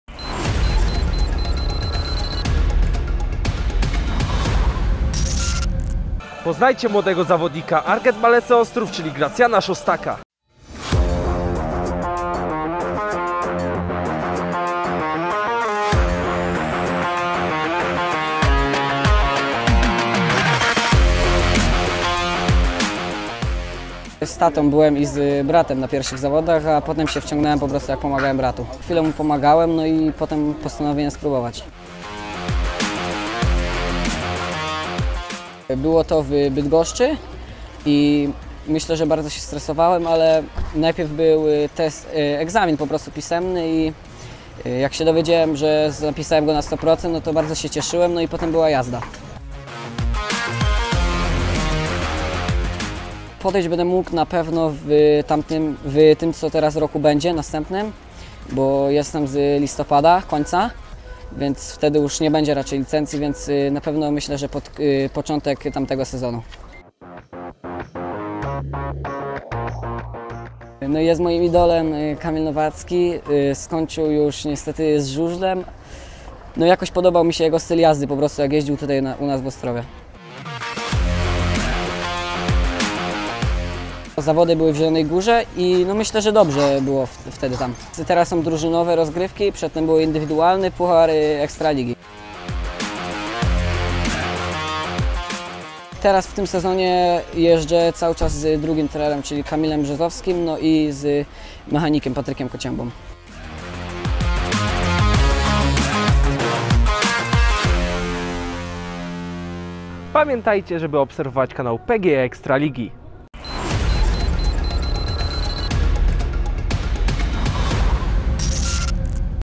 Młody talent z Ostrowa, Gracjan Szostak o początkach kariery żużlowej
Podopieczny trenera Mariusza Staszewskiego jeżdżący obecnie jeszcze na 250cc, Gracjan Szostak opowiedział o początkach swojej przygody z żużlem, licencji na 500cc i żużlowych idolach.

- Na pierwszych zawodach byłem z tatą. Później się wciągnąłem jak pomagałem bratu. Pomagałem mu przez pewien czas i postanowiłem spróbować swoich sił - powiedział Gracjan Szostak.

The adept of coach Mariusz Staszewski who is still riding on 250cc, Gracjan Szostak told about beginnings of his adventure with speedway, license on 500cc and speedway idols.